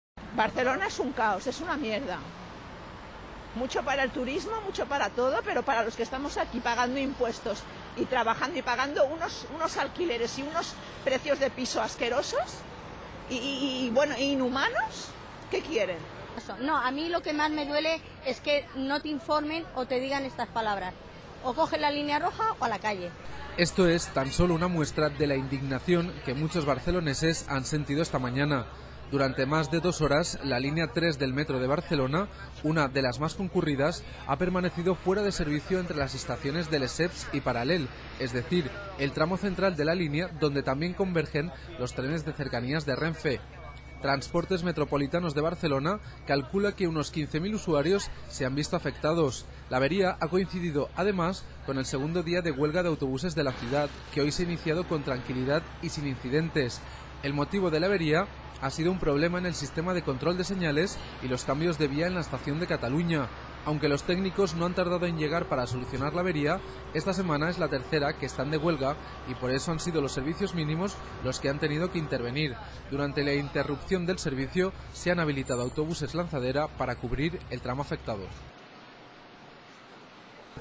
Avería en el Metro de Barcelona afecta 15000 usuarios
Barcelona, 4 mar (EFE TV).- La línea 3 del Metro de Barcelona ha restablecido por completo su servicio a las 10.29 horas después de haber quedado interrumpida la circulación durante algo más de dos horas entre las estaciones de Lesseps y Paral·lel debido a un fallo informático y que ha afectado, según Transportes Metropolitanos de Barcelona, a 15.000 usuarios. Avería. Metro. Barcelona. Línea 3.